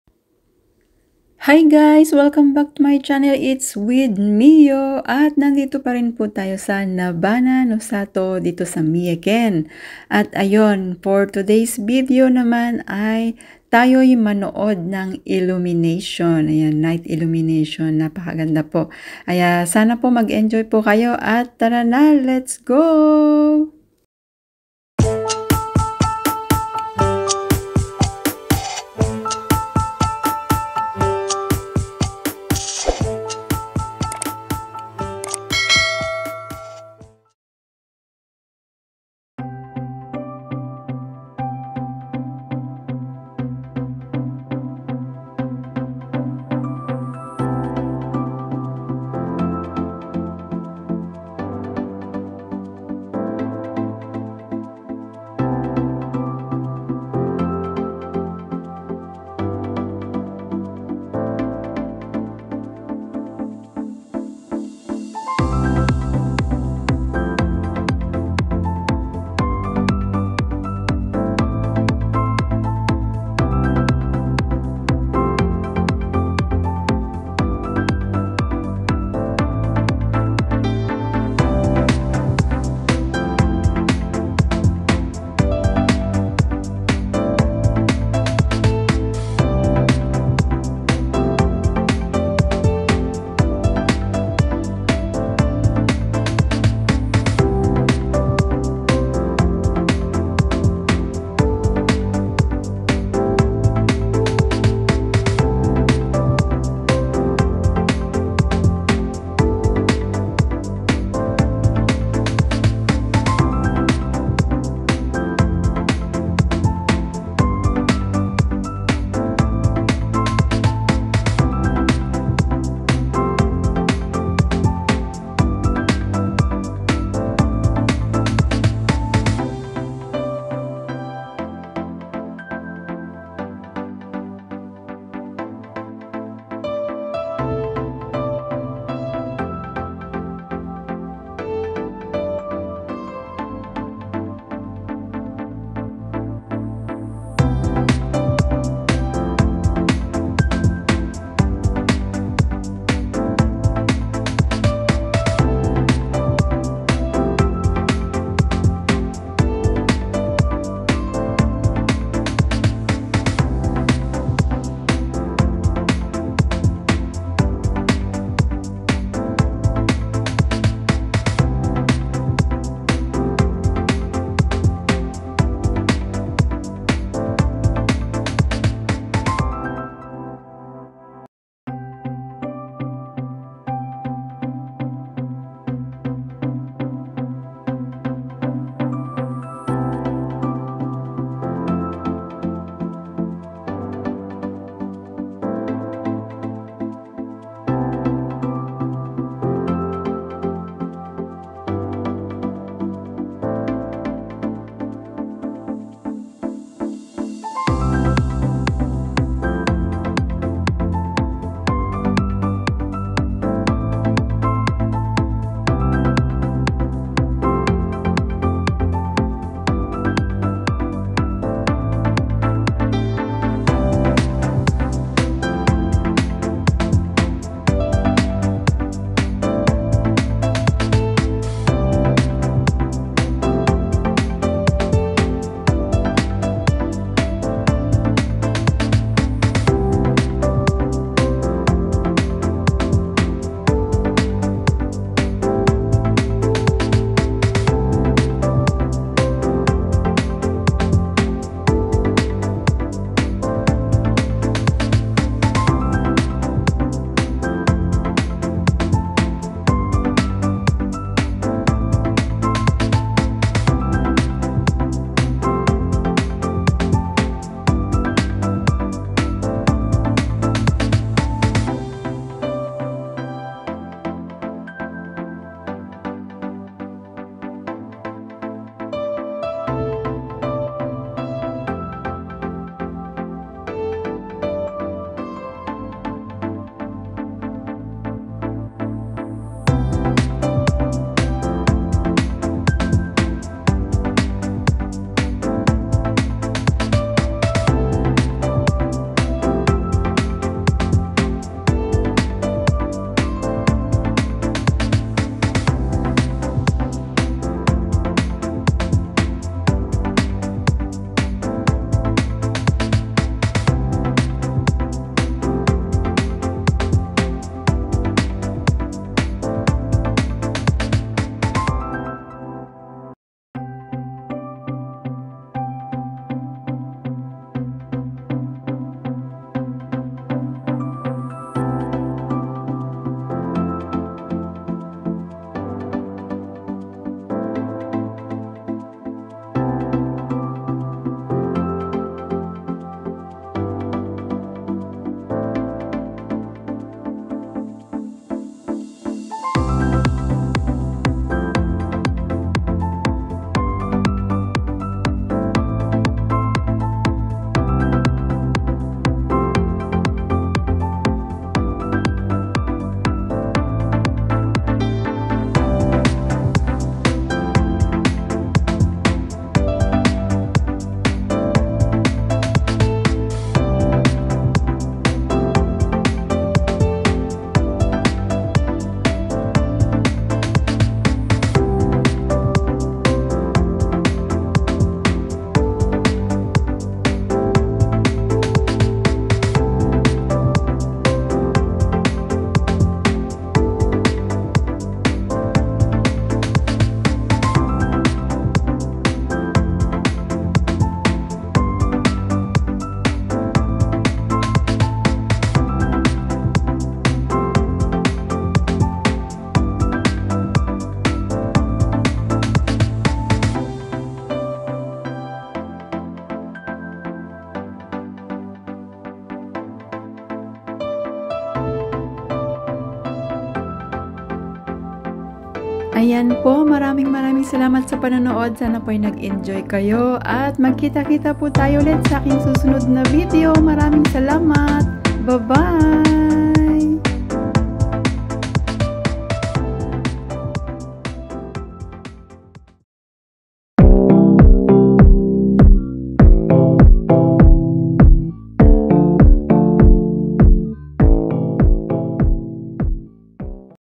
Illumination at Nabana no Sato[ なばなの里イルミネーション 2023 ] Kasama ko ang bakasyonista😊
Showing you all the beautiful places, food, events also my hobbies and my japan life.